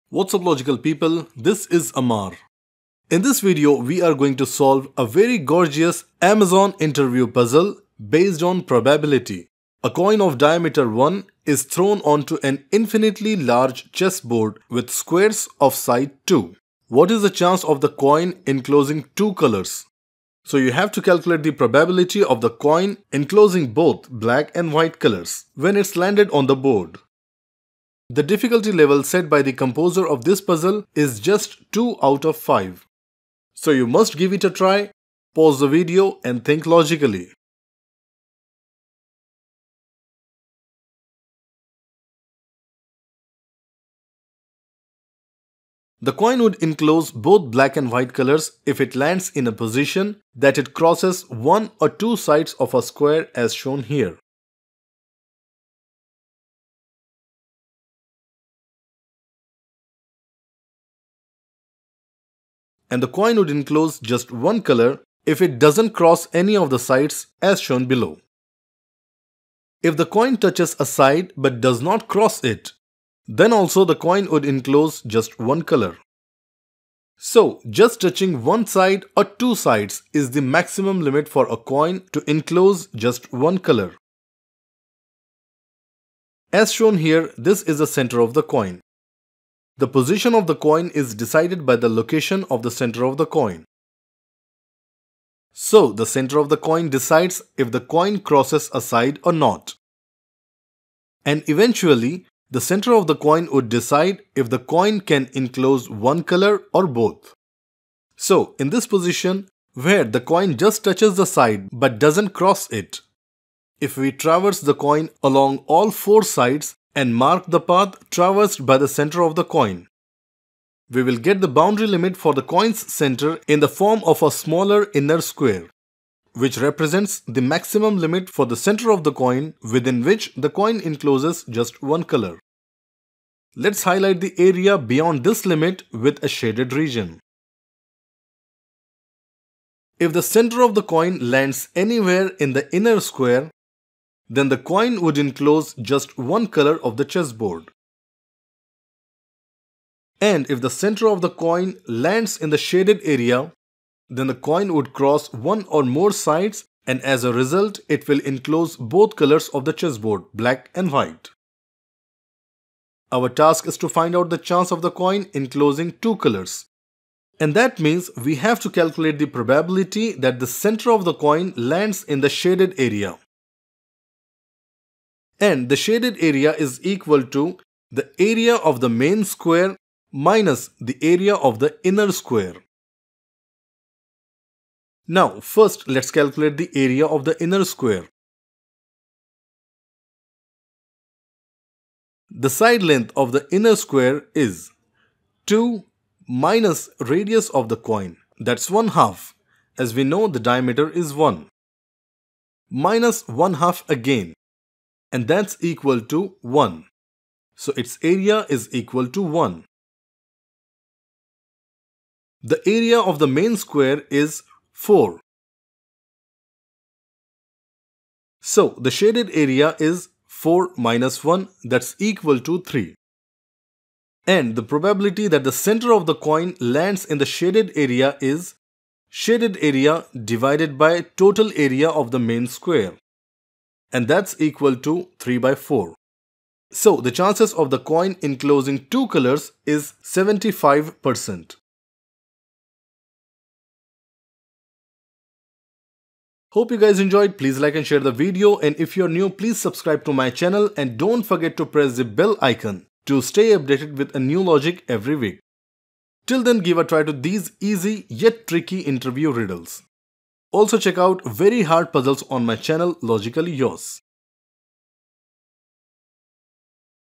Amazon Interview Question - Coin on a Chess Board - Probability Puzzle
Amazon interview puzzle:
A coin of diameter 1 is thrown on to an infinitely large chessboard with squares of side 2. What is the chance of the coin enclosing two colours? So, you have to calculate the probability of the coin enclosing both black and white colors when it's landed on the board.
The intention of mentioning an Infinitely large chessboard is that we should not consider the edges of the board, rather we should just focus on the main board.
The difficulty level set by the composer of this puzzle is just 2 out of 5.
So, you must give it a try. Pause the video and think logically.

Also try these brilliant mind blowing puzzles:

Riddle : How many coins do you need to measure height of Burj Khalifa || Think out of the box